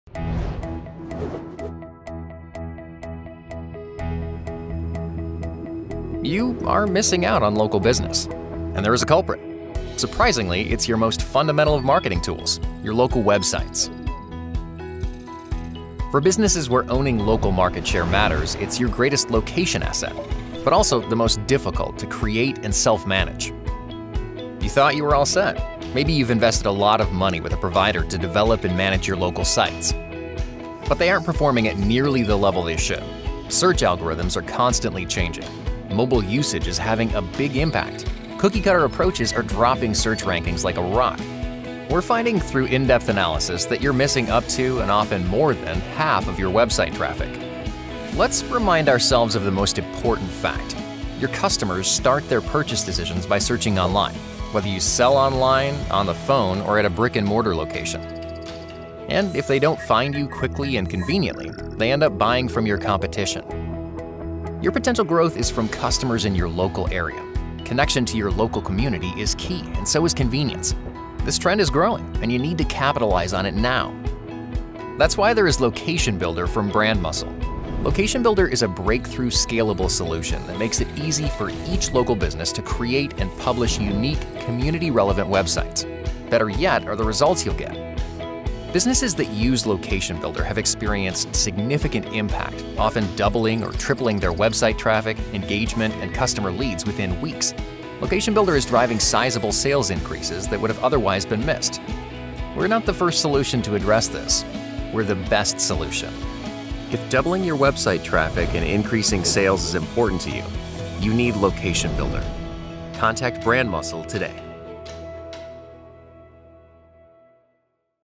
Brandmuscle LocationBuilder
LocationBuilder is an industry-leading local consumer engagement platform that enables local businesses to create and manage their location-specific microsite and/or campaign landing page, establishing a unique local presence and driving marketing performance, while maintaining brand compliance.